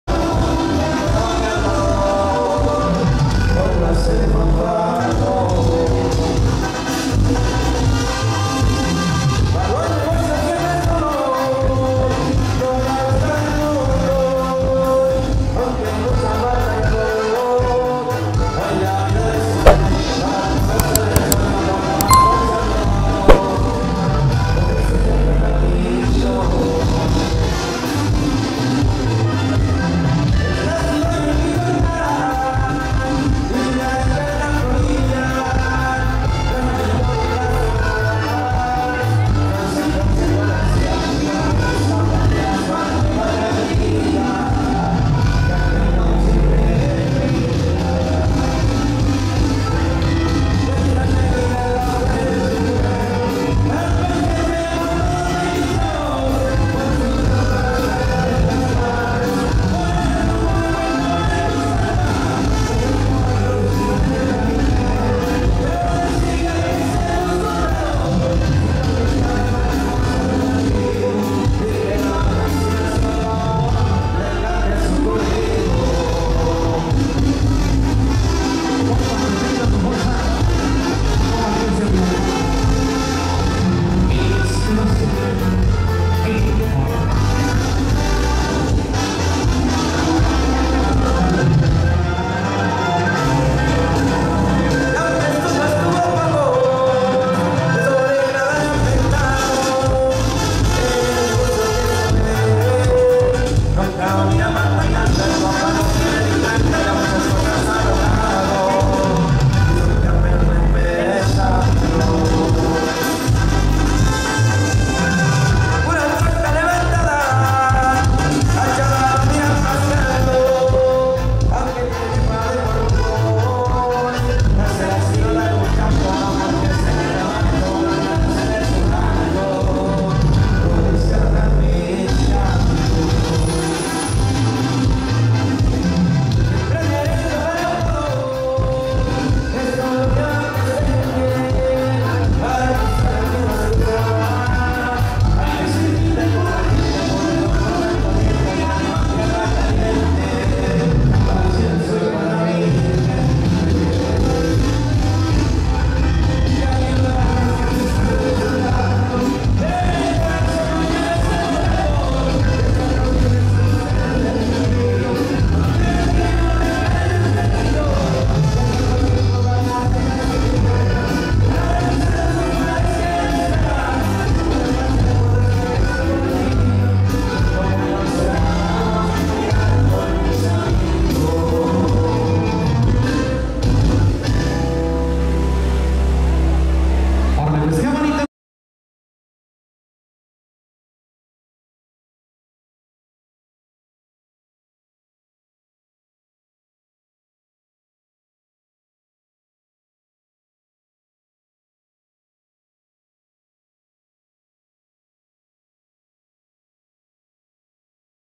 La REINA de las fiestas de PASO de TC lucio DIVINAMENTE ESPECTACULAR 💃
Bienvenidos a mi canal gente bonita aqui les dejo este bailazo en Paso de Tierra Caliente, Michoacan💃🎀😍📹🎞